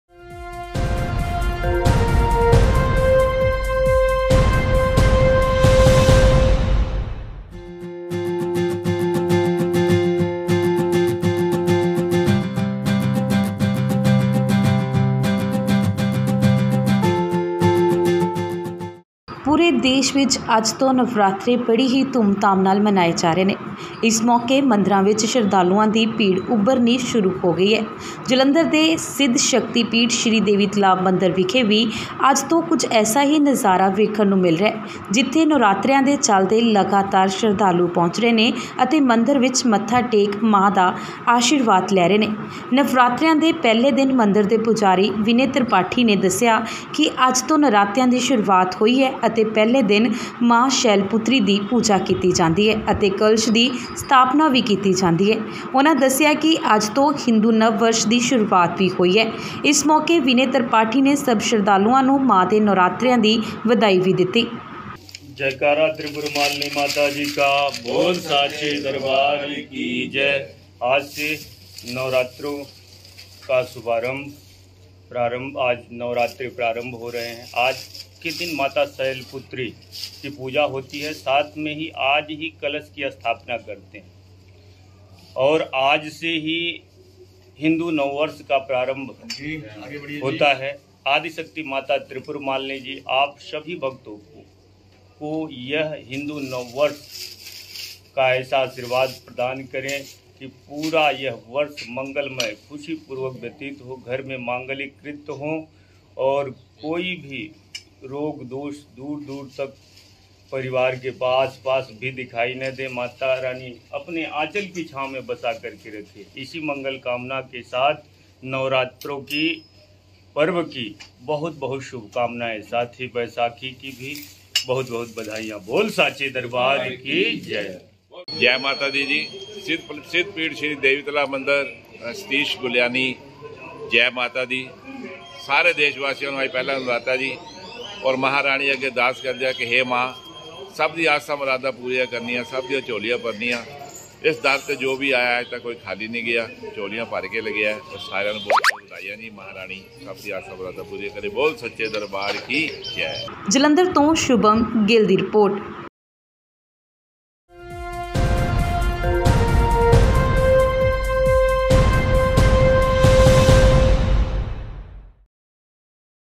ਨਰਾਤਿਆਂ ਦੀ ਹੋਈ ਸ਼ੁਰੂਵਾਤ, Jalandhar ਦੇ Shri Devi Talab Mandir ਵਿੱਚ ਲੱਗਿਆ ਰੌਣਕਾਂ
Hanesh Mehta
Editor

Kirti Gill
News Head